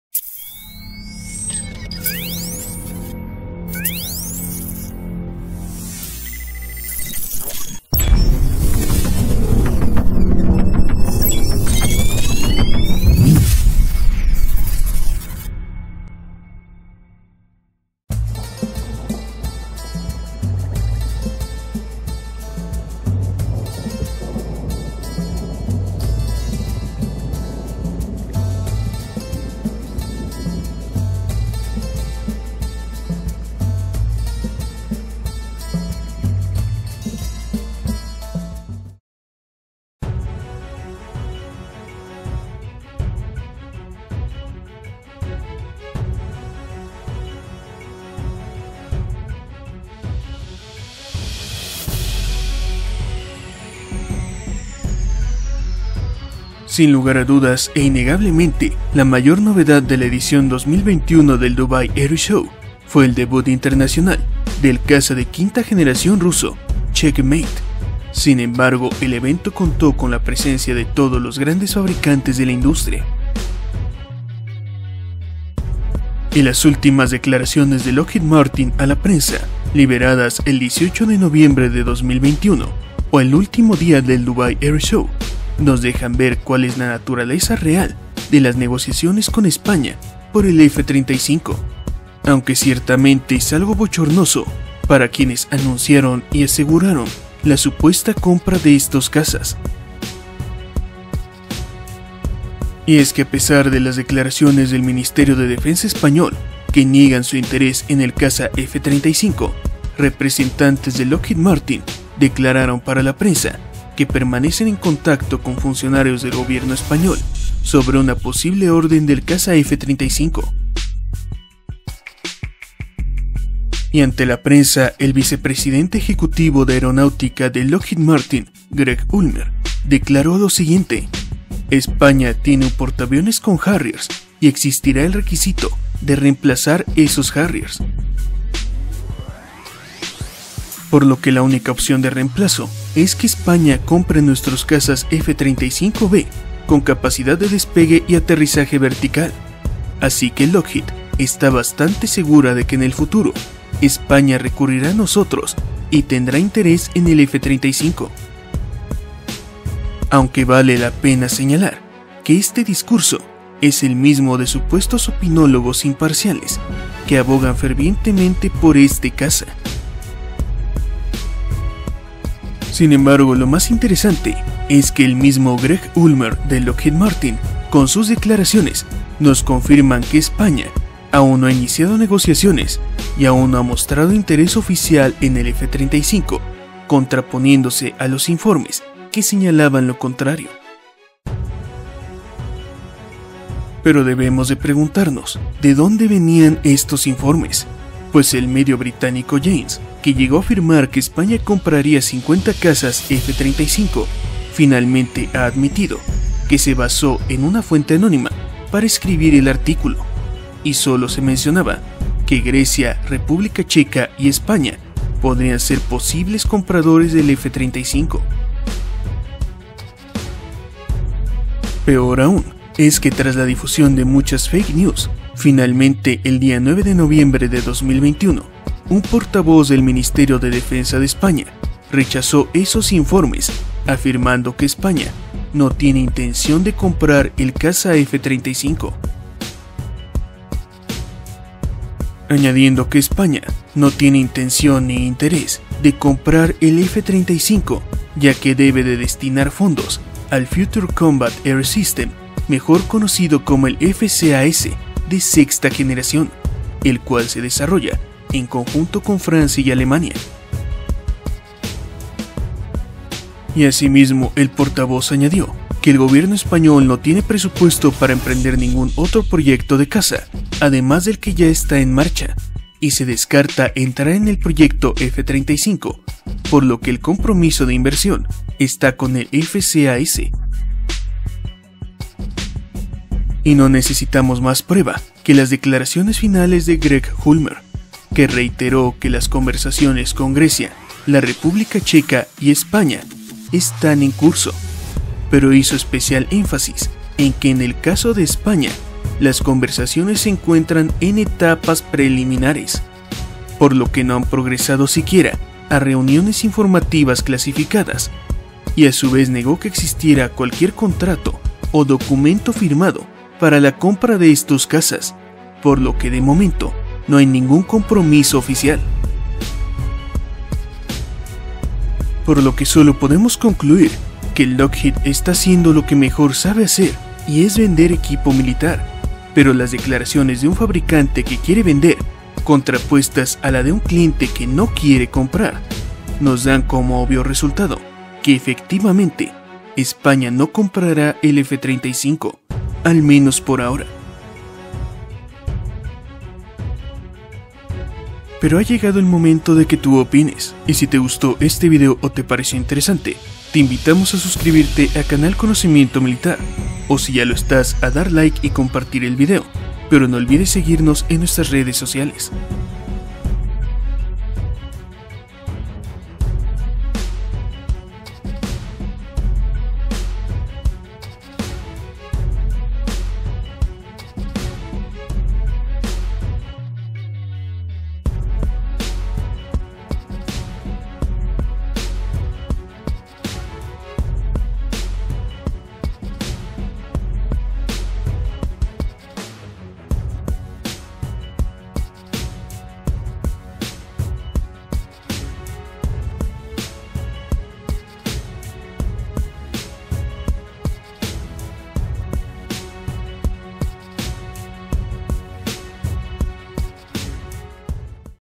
Lockheed Admite que España NO Tiene Interés en el F-35
Durante el Dubái Air Show 2021, el Vicepresidente Ejecutivo de Aeronáutica de Lockheed Martin Greg Ulmer Admitió que España no ha tenido Interés Oficial en el F-35 y descartó que exista un Acuerdo firmado con España.